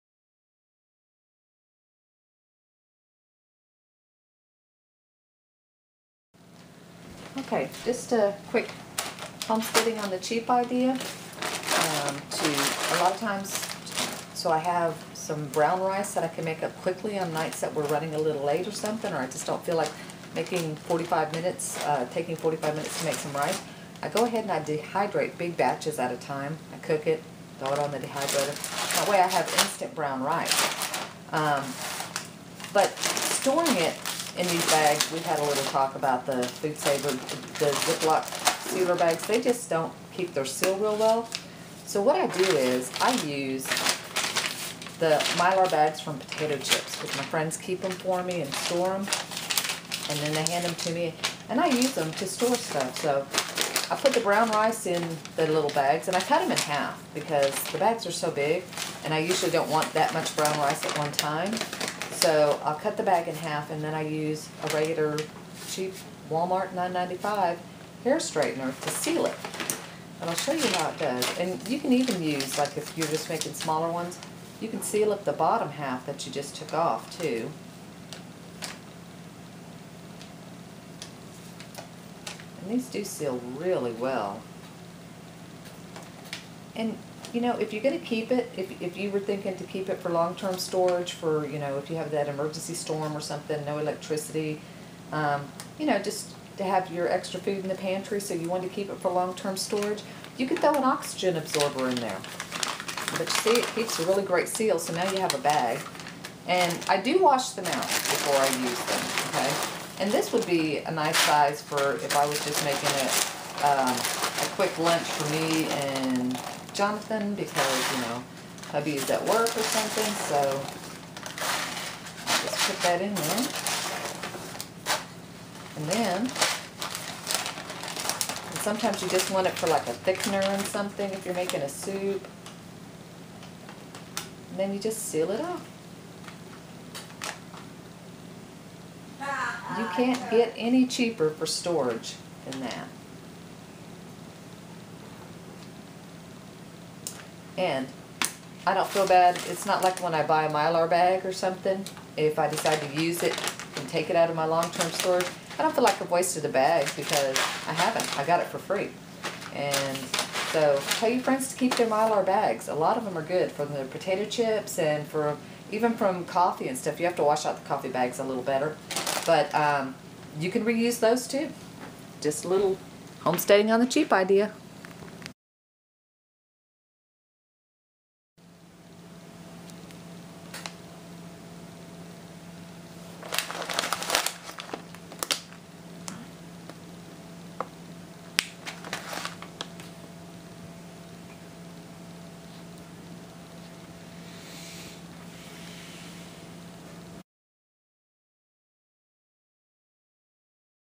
Homesteading on the cheap - Mylar Bags
A quick tip on how to reuse those chip bags that are so abundant in today's society